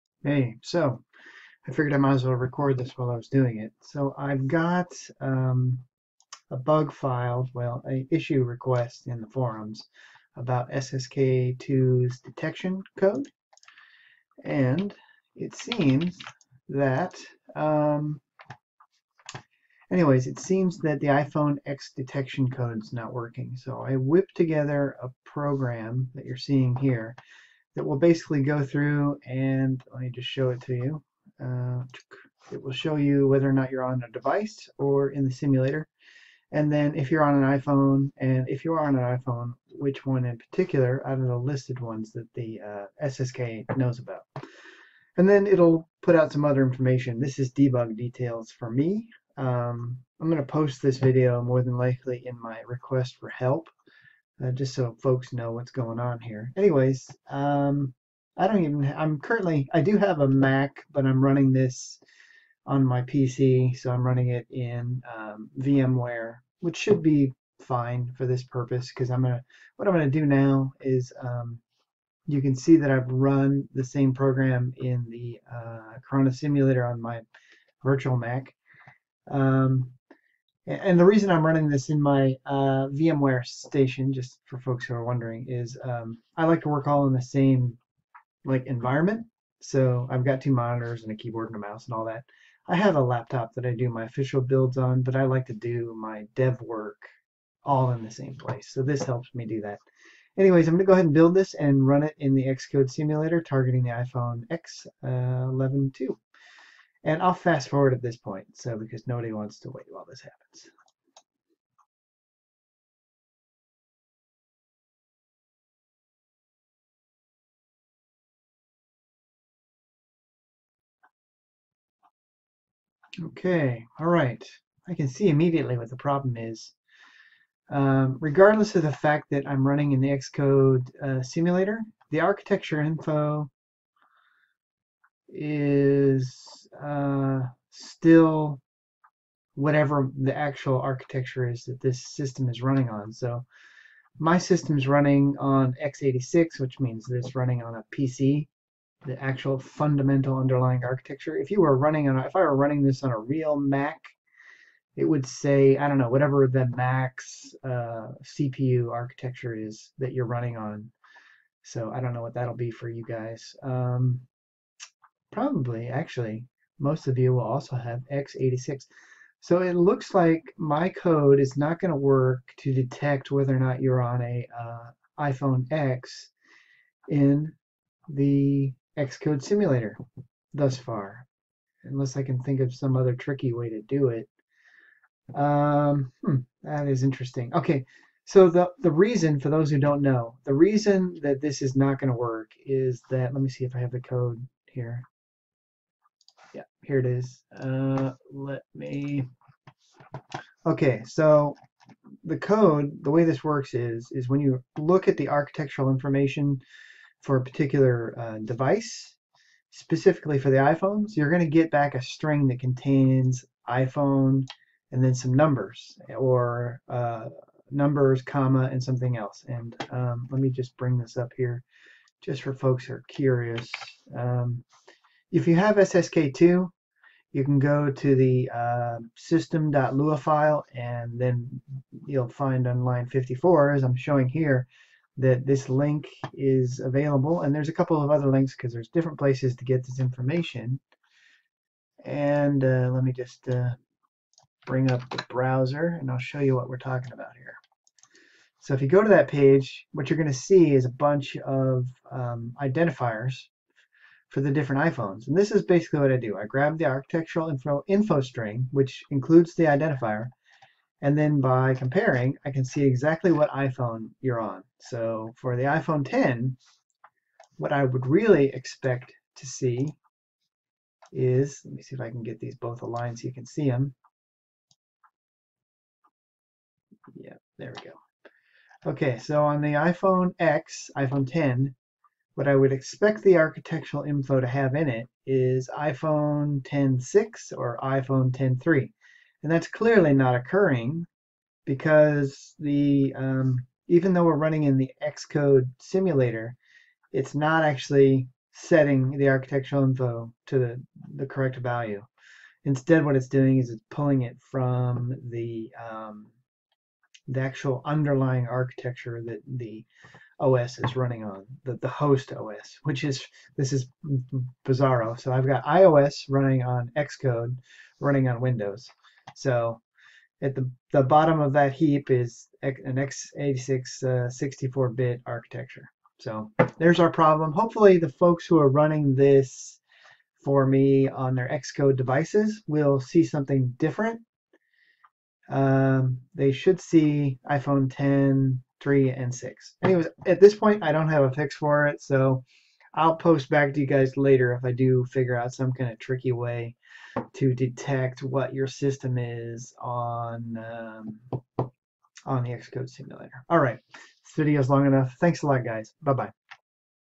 SSK2 Not Detecting iPhone X Investigation - xCode Simulator
I got a question about SSK2 and the fact that it seems to NOT detect that it is running on the iPhoneX.

This was a two part issue.  One, the issue occurred in the xCode simulator, and Two, the user said it was also happening on real devices.

I believe the prior, but I doubt the latter.  However, not owning a iPhoneX, I am waiting on help from other testers.